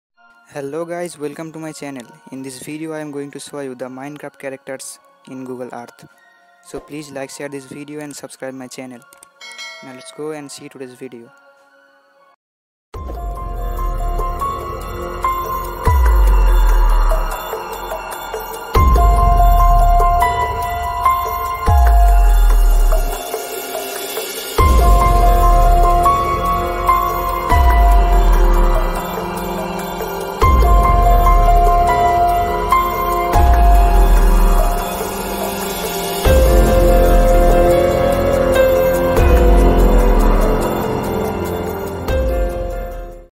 Omg!😍 the Minecraft🤑🤑 found in Google Earth / I found the Minecraft character in Google Earth
Hello my friends welcome to my channel this video is about the minecraft character I found him  in Google Earth if you are interested to saw more mysterious videos about Google Earth just subscribe my channel and press the bell icon 🙏🔔

Thanks you guys for watching this video 🙏